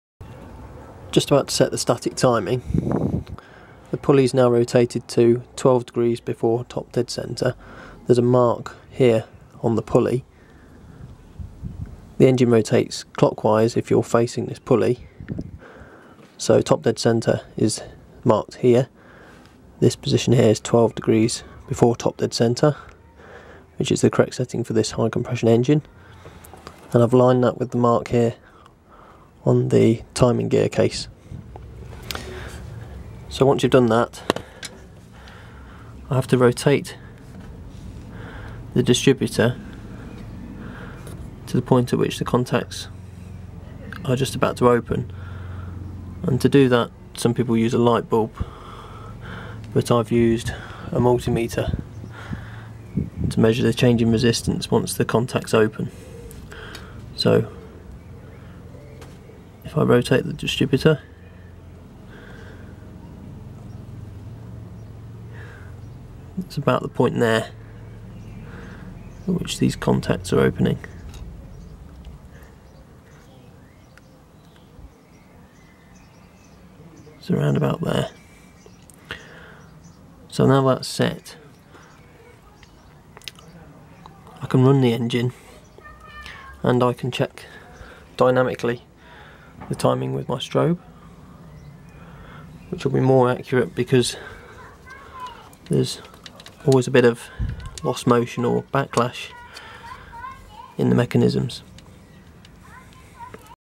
31. Triumph Herald 948 engine rebuild - Setting static ignition timing
Setting ignition timing to 12 degrees Before Top Dead Centre (BTDC)